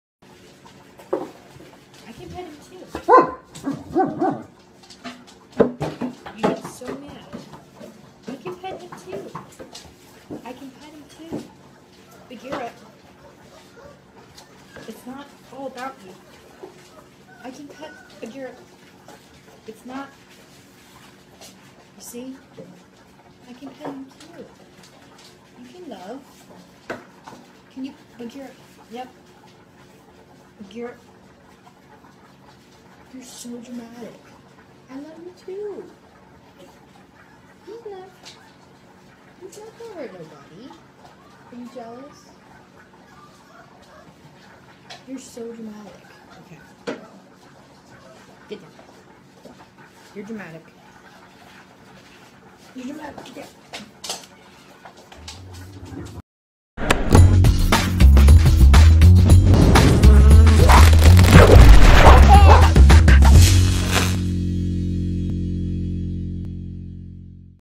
Great Dane extremely jealous of new puppy
This dog owner shows us just what happens when she wants to pet her new puppy. An enormous and envious Great Dane jumps on her, attempting to come between her and the puppy. This has got to be the epitome of dog jealousy!